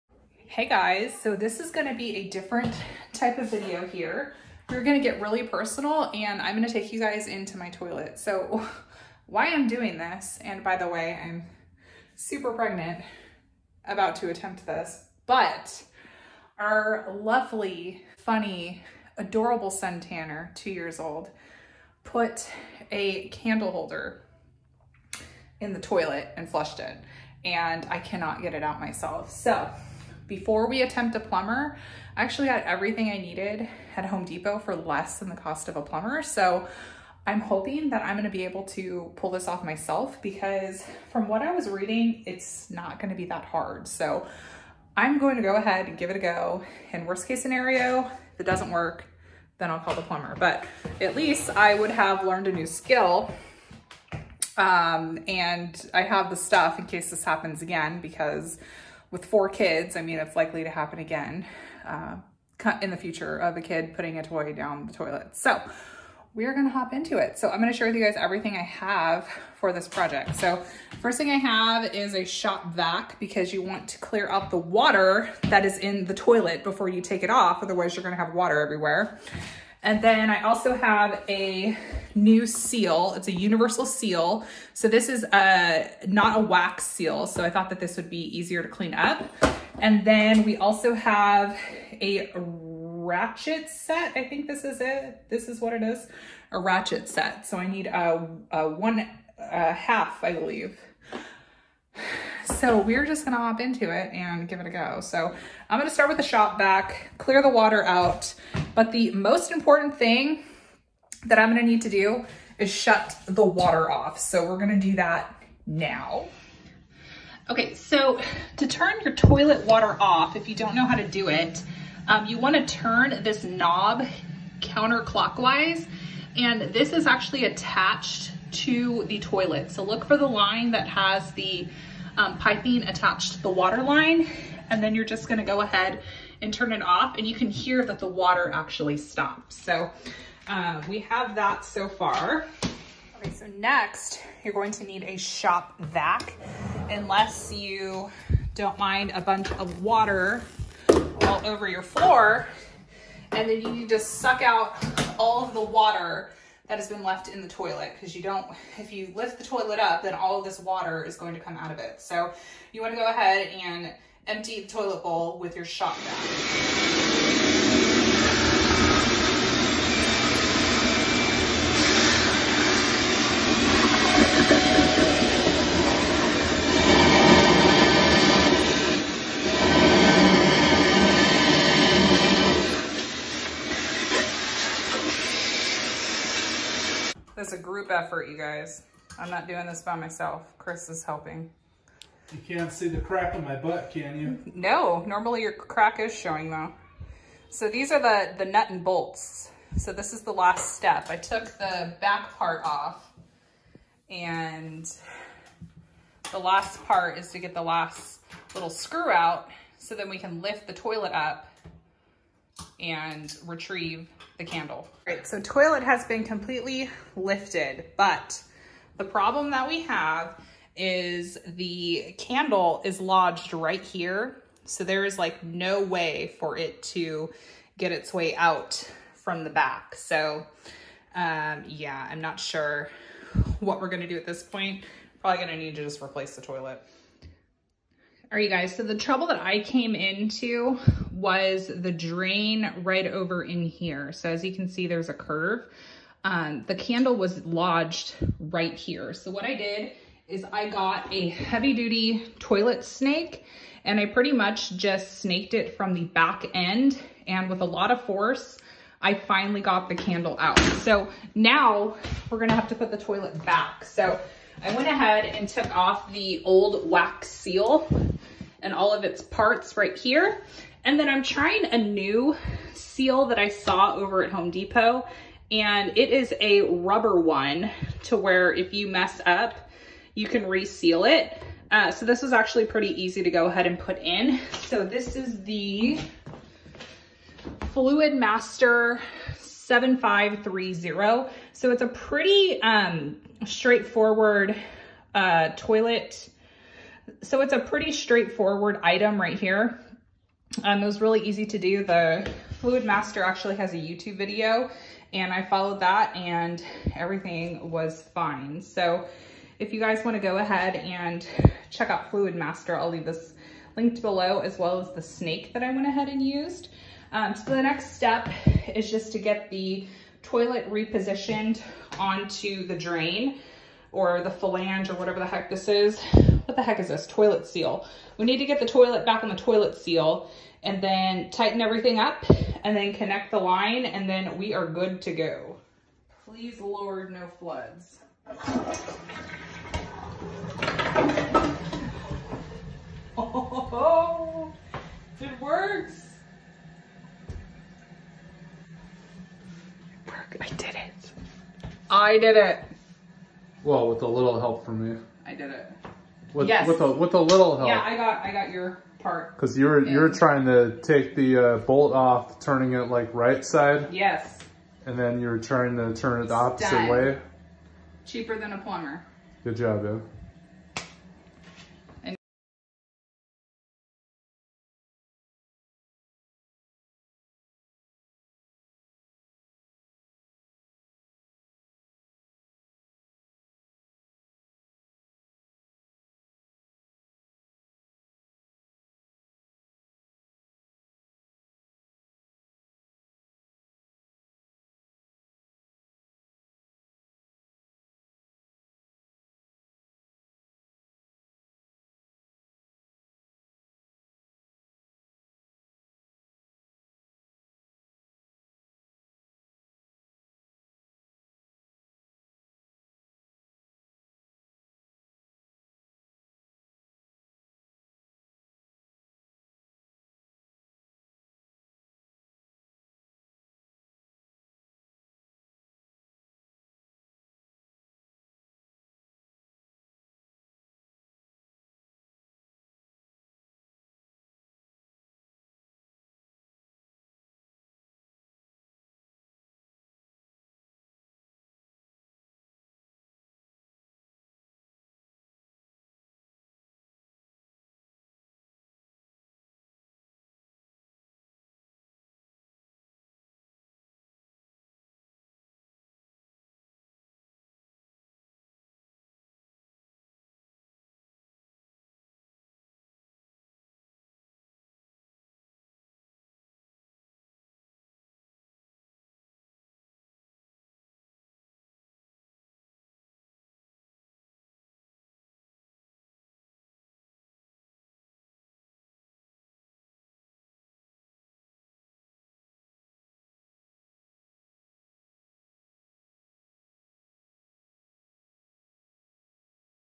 How to Remove a Foreign Object From A Toilet | No Plumber Needed
In this video I am sharing with you all how I removed an object stuck in my toilet.

Materials I used:
Fluid Master Toilet Seal

FLUZ APP-EARN UP TO 35% CASH BACK ON ONLINE AND IN STORE PURCHASES!

jqbzrb